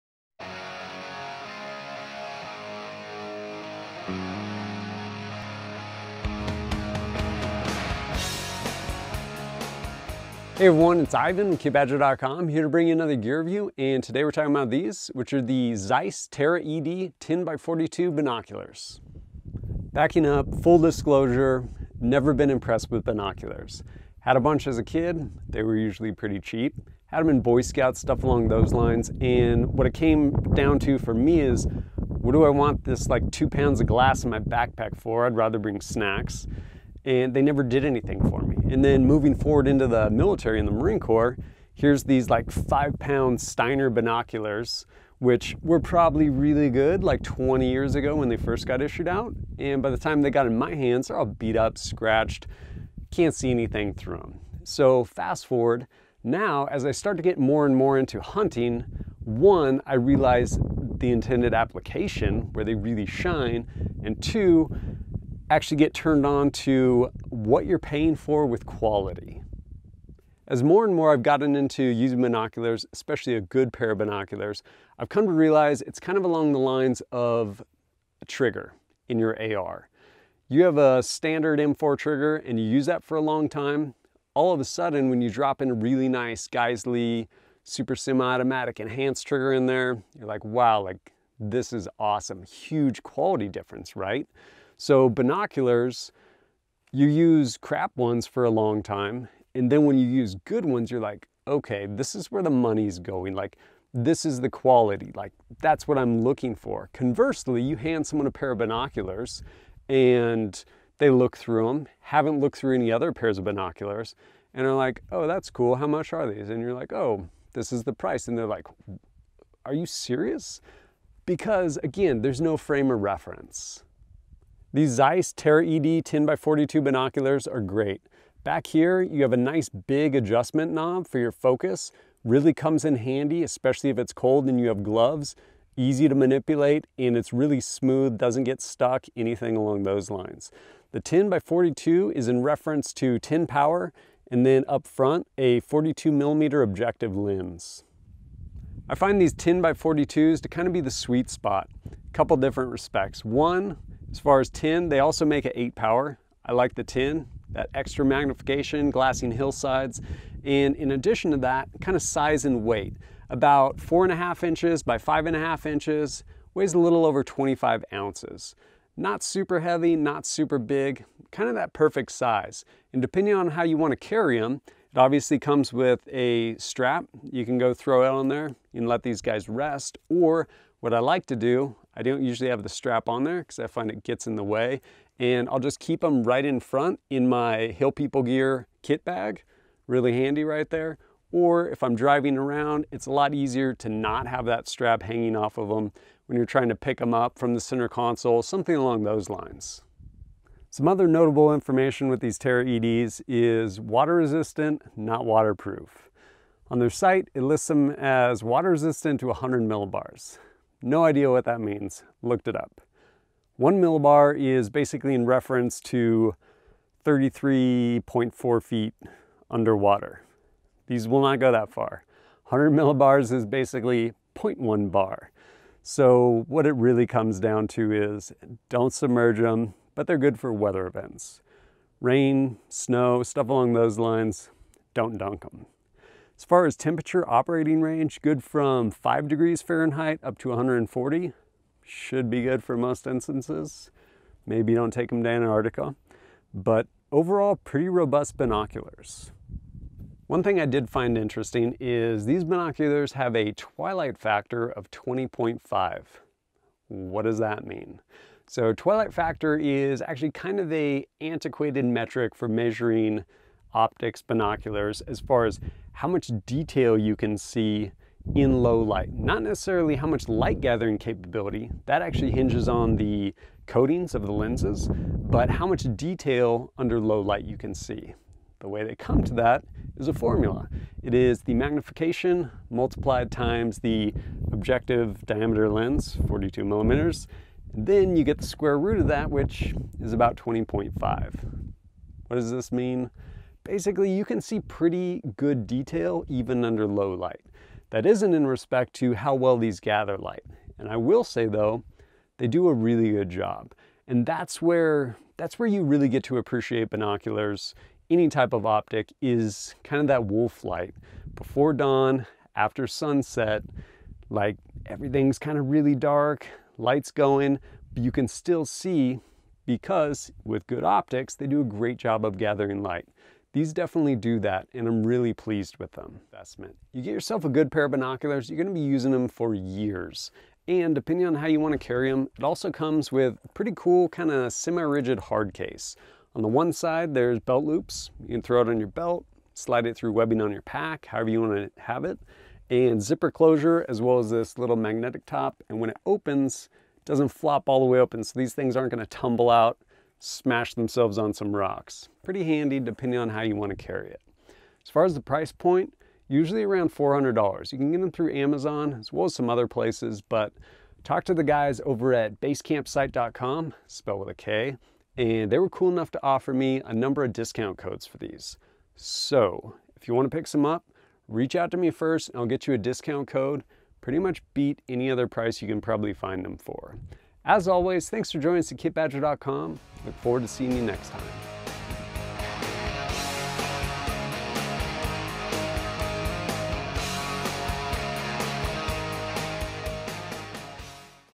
Zeiss Terra ED 10x42 Binoculars
Good optics make a huge difference...  Like most pieces of gear, it takes perspective to appreciate the quality.  These Zeiss Terra ED 10x42 binoculars are pretty amazing at their price point.  Incredible clarity, great light transmission in low light situations and all in a pretty compact package.